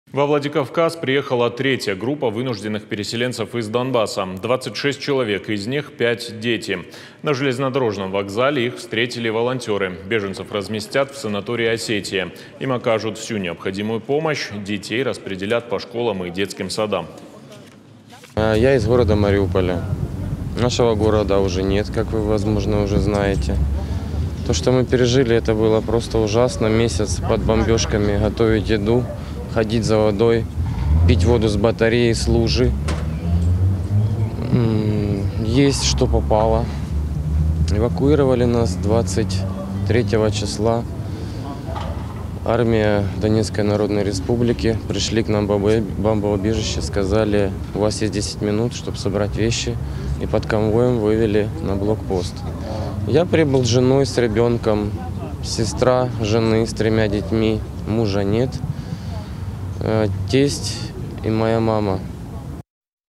Во Владикавказ приехала третья группа вынужденных переселенцев из Донбасса
Во Владикавказ приехала третья группа вынужденных переселенцев из Донбасса. 26 человек, из них 5 - дети. На железнодорожном вокзале их встретили волонтёры. Беженцев разместят в санатории «Осетия». Им окажут всю необходимую помощь. Детей распределят по школам и детским садам.
Мы рядом в:
ok.ru/irystontv